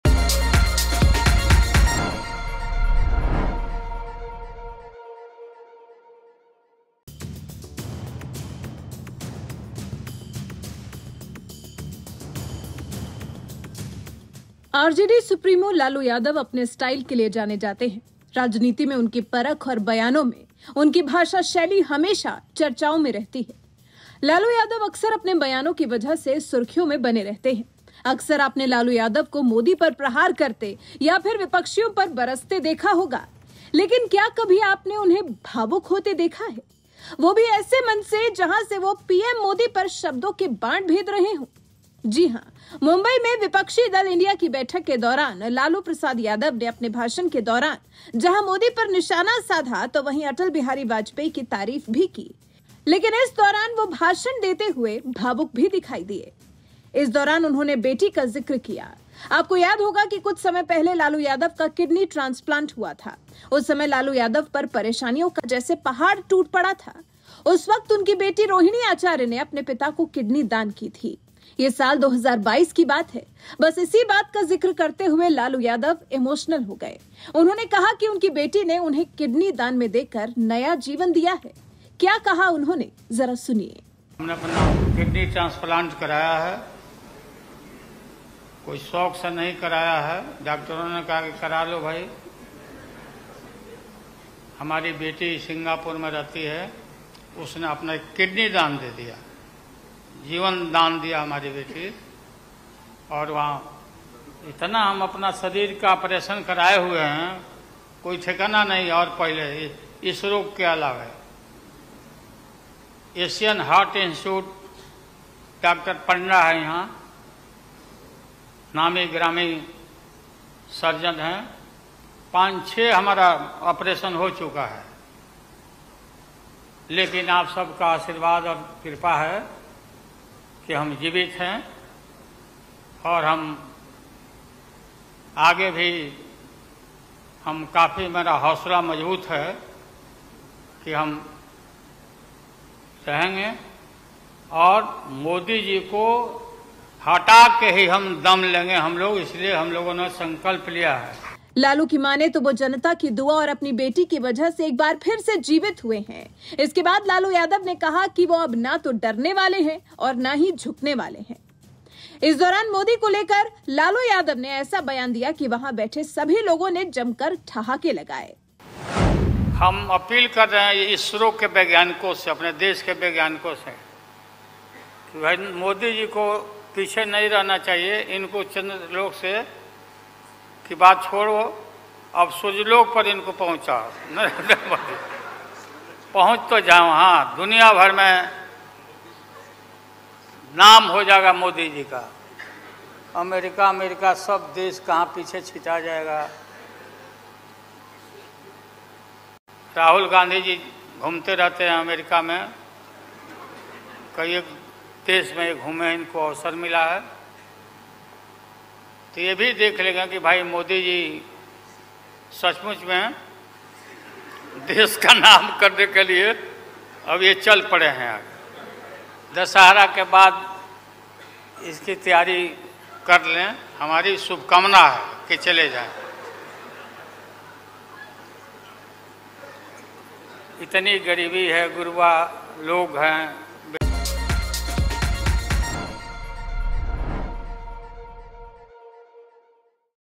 मंच पर दे रहे थे भाषण, एक पुरानी याद को लेकर सभी के सामने भावुक हो गए Lalu Yadav
आरजेडी सुप्रीमो लालू यादव अपने स्टाइल के लिए जाने जाते हैं राजनीति में उनकी परख और बयानों में उनकी भाषाशैली हमेशा चर्चाओं में रहती हैं…लालू यादव अक्सर अपने बयानों की वजह से सुर्खियों में बने रहते हैं..अक्सर आपने लालू यादव को मोदी पर प्रहार करते या फिर विपक्षियों पर बरसते देखा होगा..लेकिन क्या कभी आपने उन्हें भावुक होते देखा है.

Jogi
Rakhi